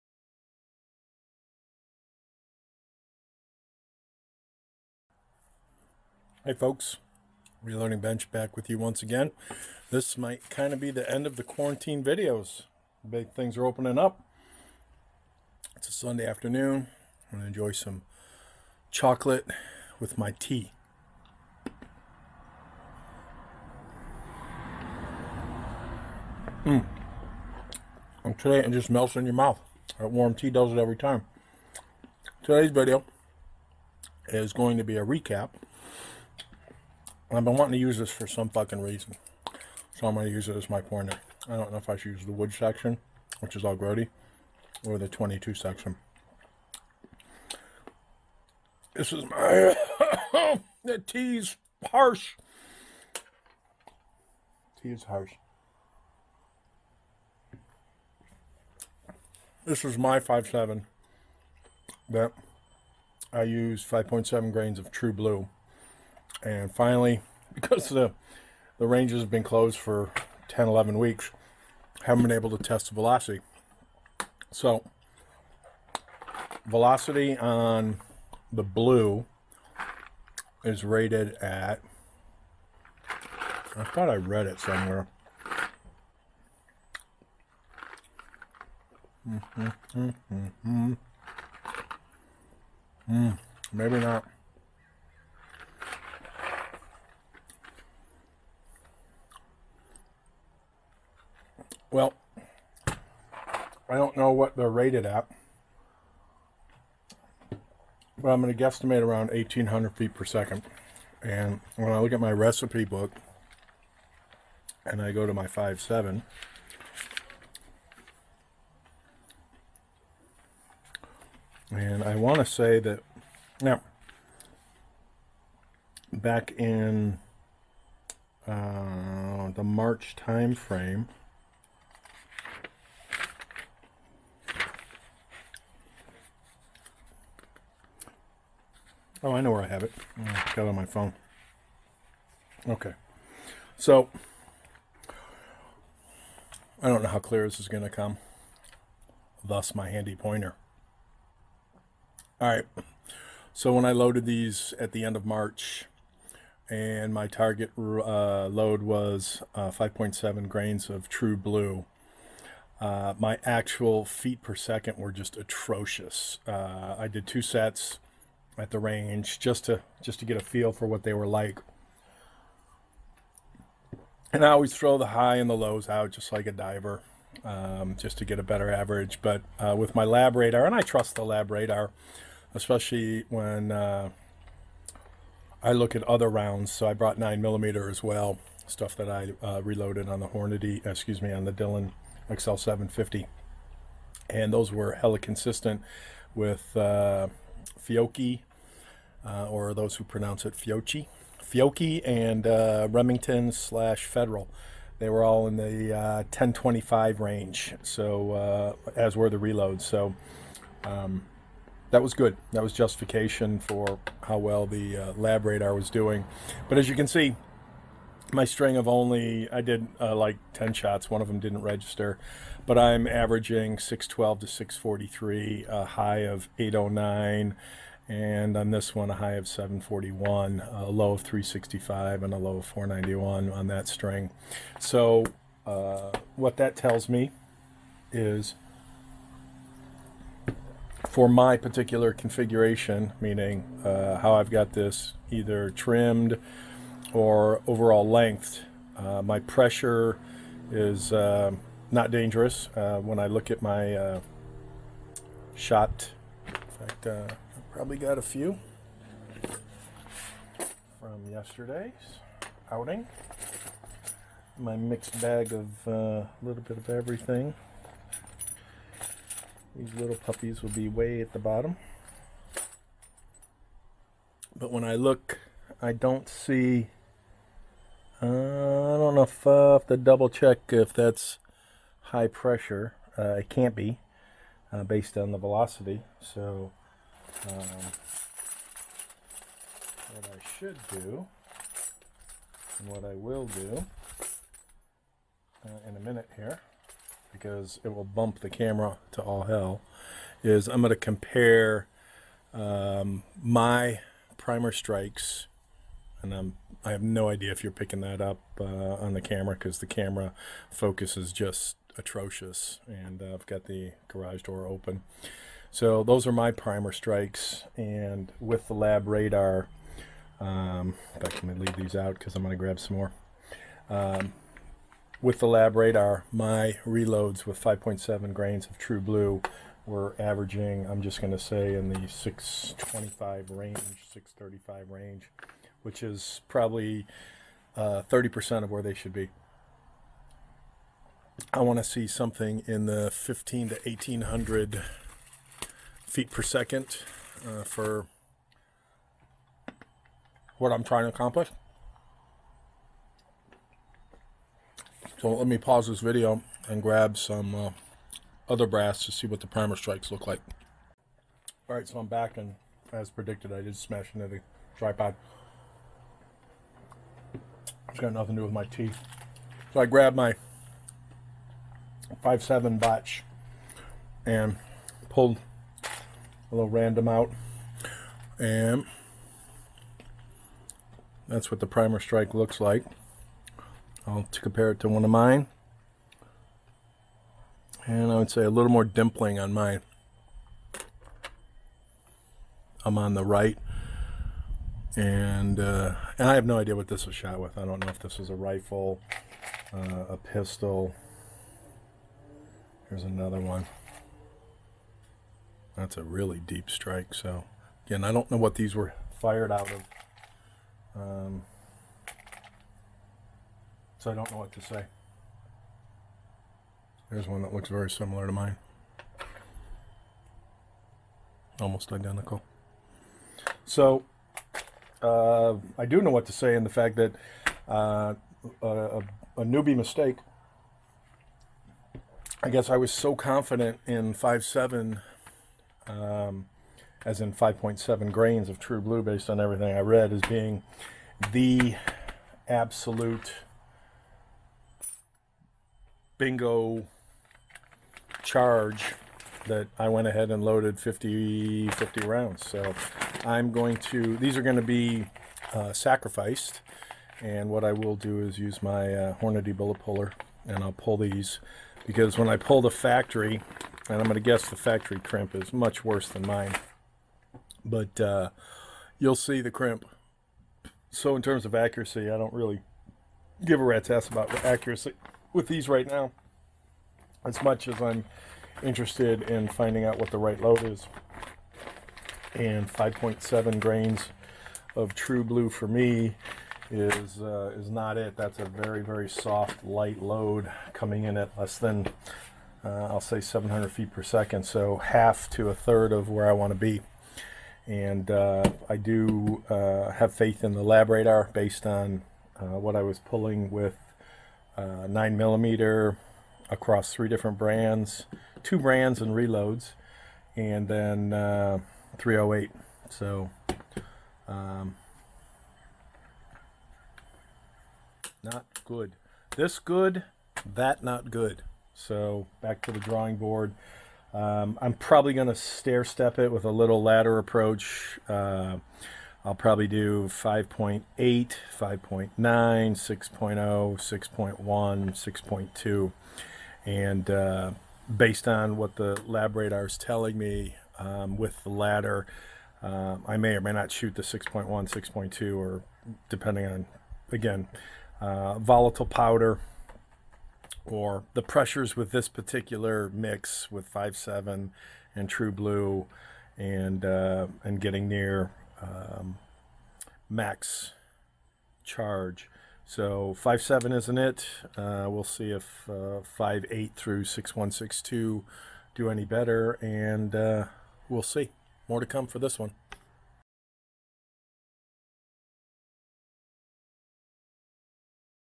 Reloading for the 5.7x28 Pistol Round: FPS Velocities @ the Range (Surprising Results)
Now that the local range finally opened up once again, I decided to test my FN 5.7x28 reloads.  As you can see from the video, I was rather surprised and somewhat disappointed at the results.  My load was apparently a bit too light.

Back in February, when I tested the factory loads with my Labradar, I was getting a low of 1637 and high of 1701 FPS for a velocity average of 1673 FPS.  Totally what I would expect.

My charge weight of True Blue gave me MUCH lower velocities, almost 2/3 slower on average.  Made a total newbie mistake by reloading 50 rounds based on how confident my charge was from what my research "told me".  Goober.  Should have gone back to basics and created a ladder set of five rounds each of 5.7, 5.8, 5.9, 6.0 grains.  Oh well, time to go back to the drawing board.  I will pull the heads off these rounds and do what I should have done the first time around so that I can come close to factory velocities, with a target range of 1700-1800 FPS.

I did show my primer strikes and after checking, I would say there is some cratering, which by itself, is not a sign/indicator of over pressuring, especially considering my recorded velocities.  If I was over 1800 FPS and cratering, I'd be a bit more concerned.  Let's see how the ladder sets turn out.  More to come...